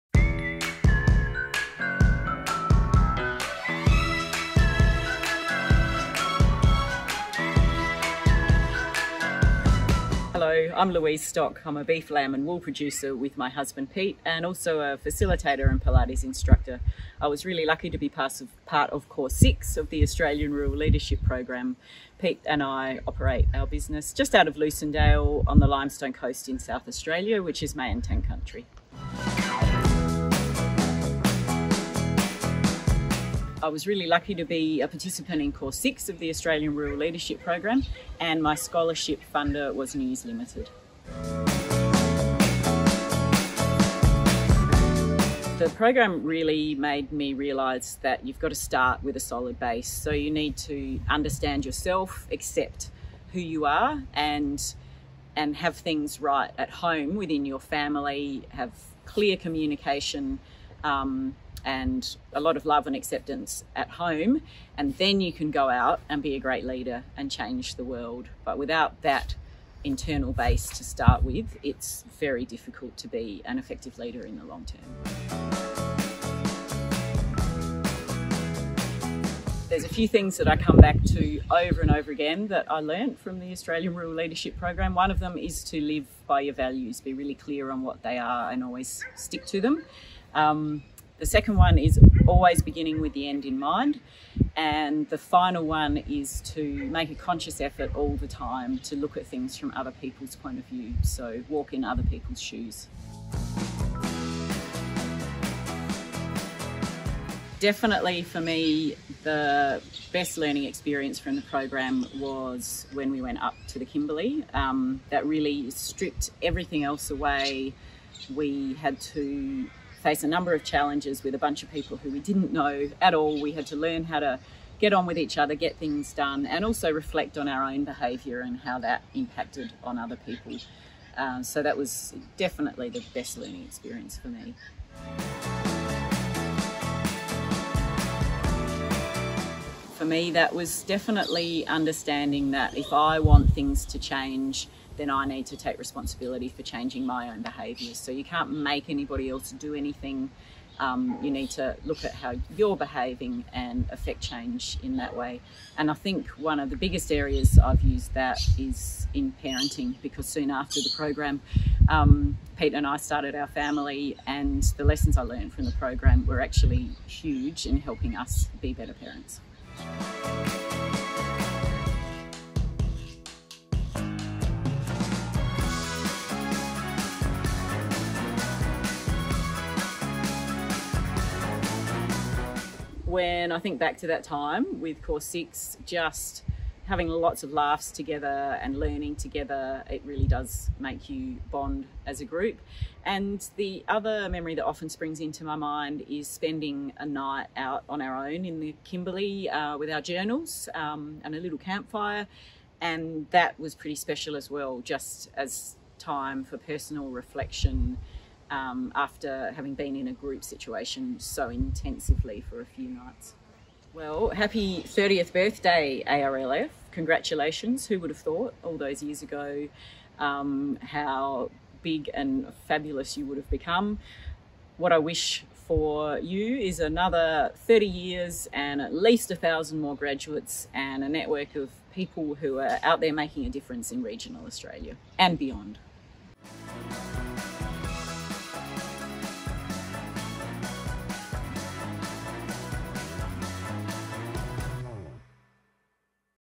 30 YARNS FOR 30 YEARS - Louise Stock
Louise Stock has many strings to her bow - she's a beef, lamb and wool producer and facilitator/pilates instructor from South Australia. She's also an Australian Rural Leadership Program alumni!

A participant in Course 6, Louise continues to return to invaluable insights and ideas she gained from the program.
Louise says, "There's a few things that I come back to over and over again that I learned from the ARLP and one of them is to live by your values. Be really clear on what they are and always stick to them. The second one is always begin with the end in mind and the final one is to make a conscious effort all the time to look at things from other people's point of view, so walk in other people's shoes."
Enjoy Louise's video in celebration of our 30th birthday!